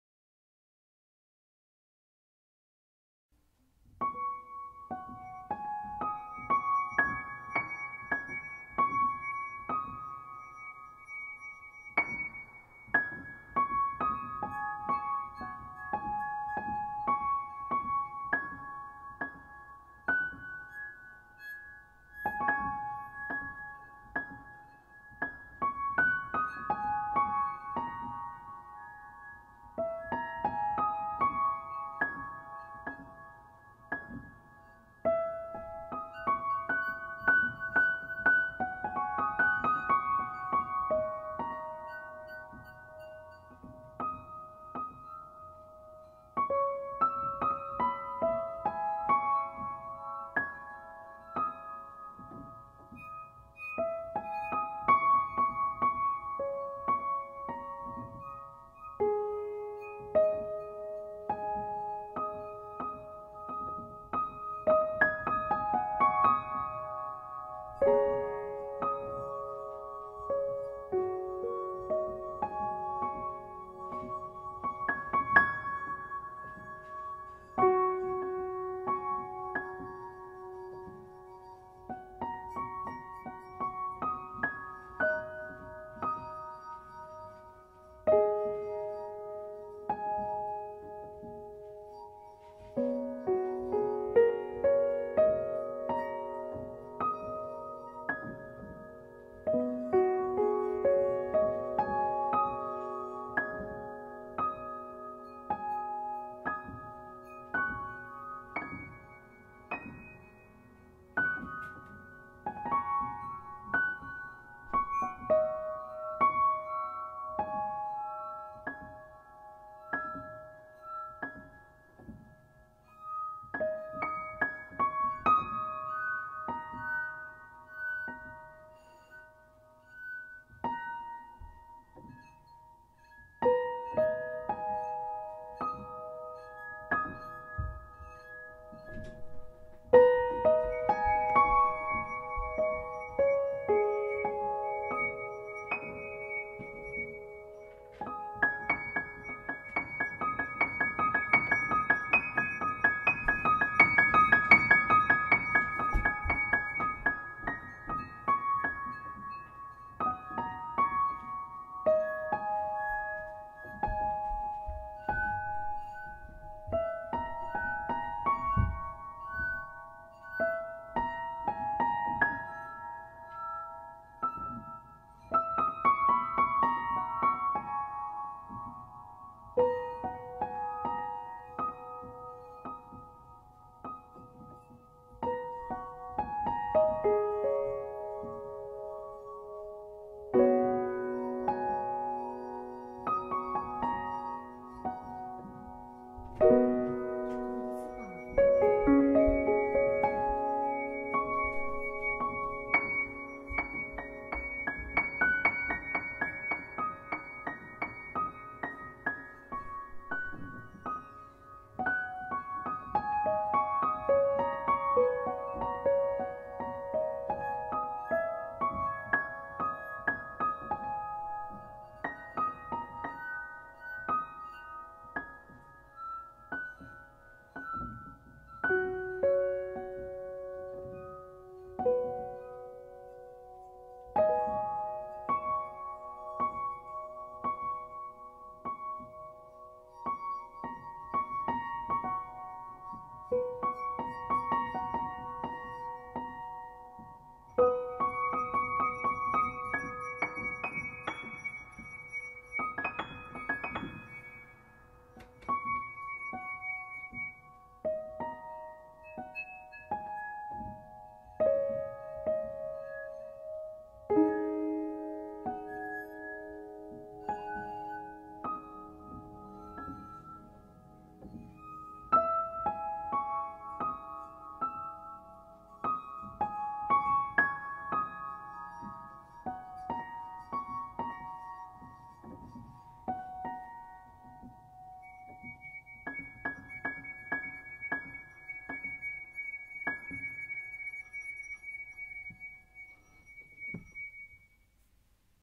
glass&piano
Every kids likes to play glasses. Sometime we enjoy this in music class.

glass Kamin

piano Serizawa Tomo